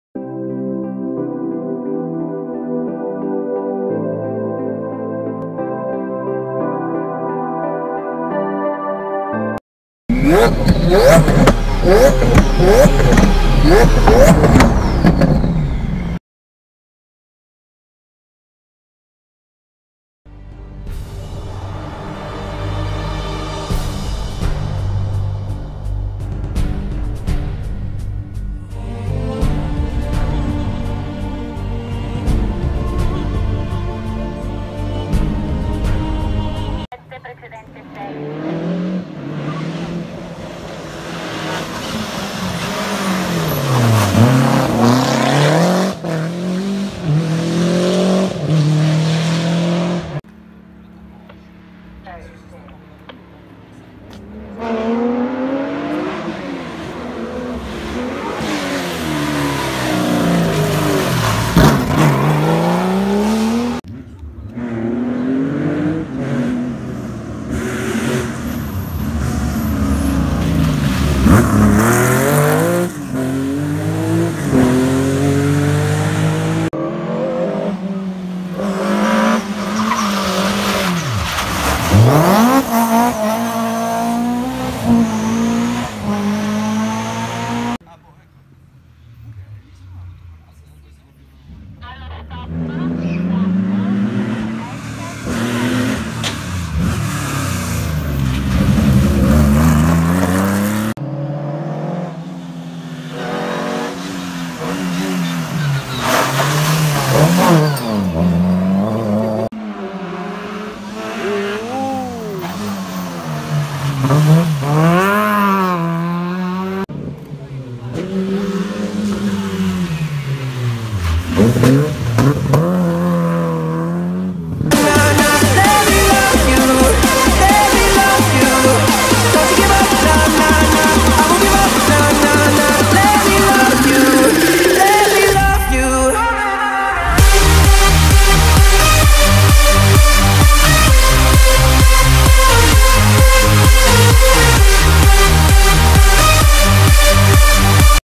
5° La Grande Corsa Chieri 2018 & ACTION (HD)
I migliori passaggi ripresi alla Grande Corsa 2018

Cars in this video:
Lancia Delta integrale gr.A
Porsche 911 RS
BMW M3 E30 gr.A
Lancia Fulvia HF 1.3
Autobianchi A 112 Abarth
Opel Manta 2.0 E

-Drone eachine e58
-Iphone 7
-Samsung J7 2016